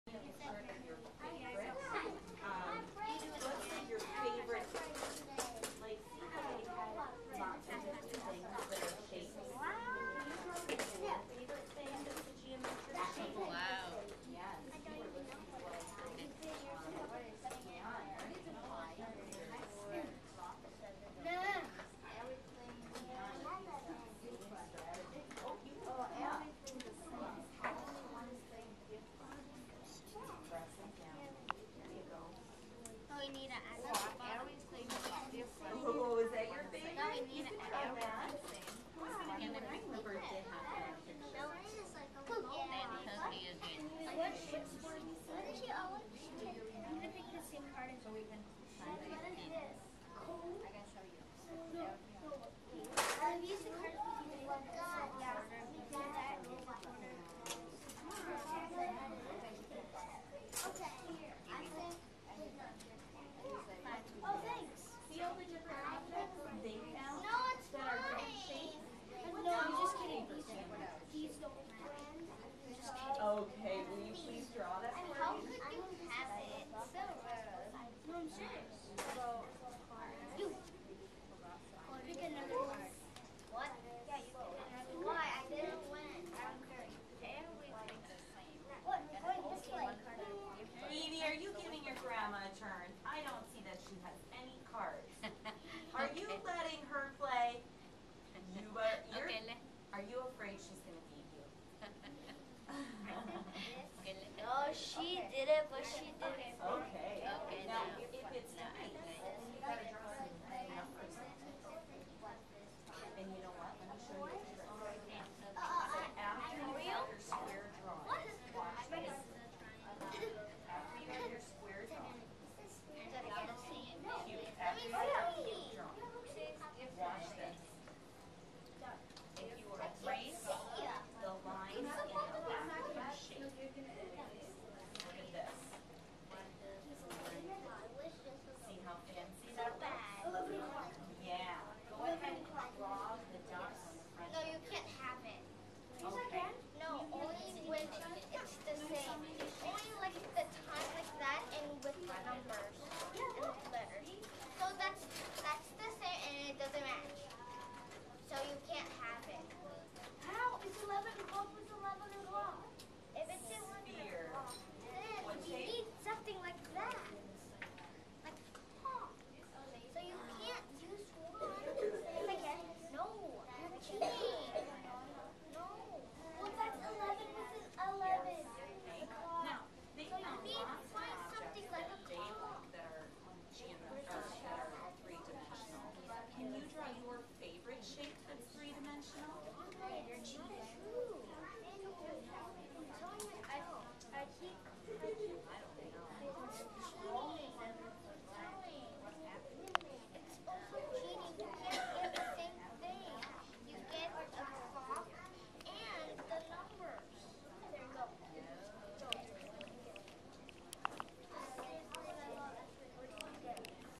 Evie's Math Worshop at SSIS 3/20/2015 I
Evie beat badly the opponent in this game